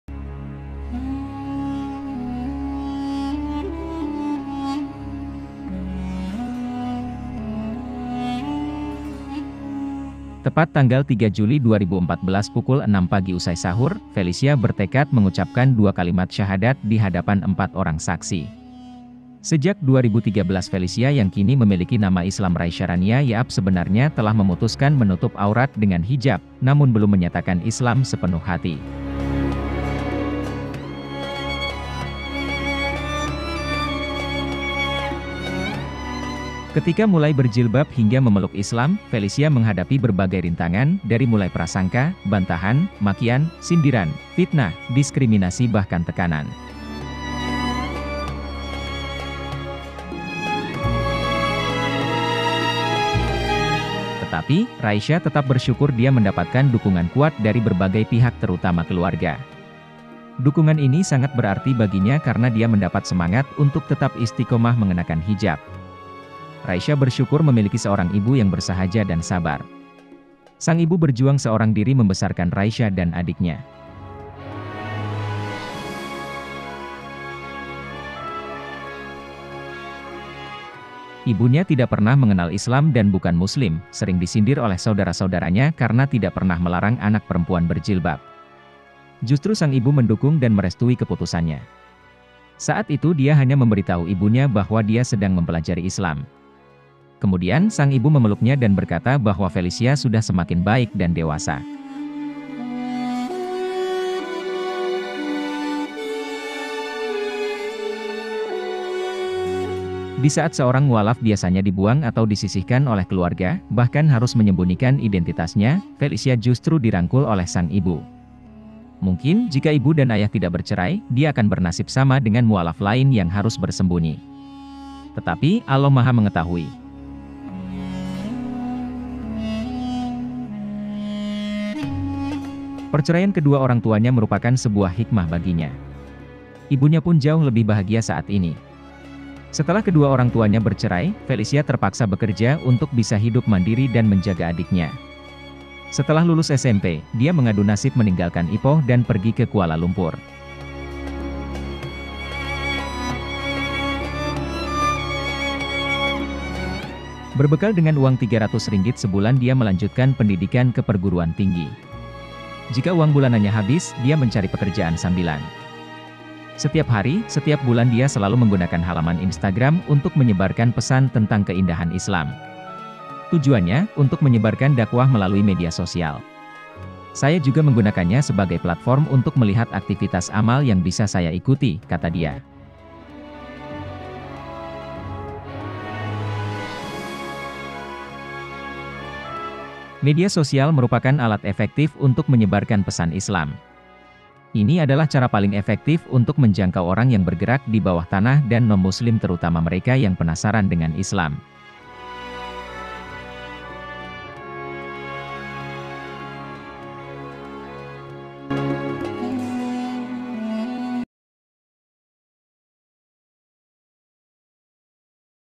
KISAH MUALAF 2021 Felixia Yeap Rela berpisah Dengan Ayah Demi Mencari Kebenaran Islam
KISAH MUALAF 2021 Felixia Yeap Rela berpisah Dengan Ayah Demi Mencari Kebenaran Islam#mualaf#kisahmualaf#mualaf2021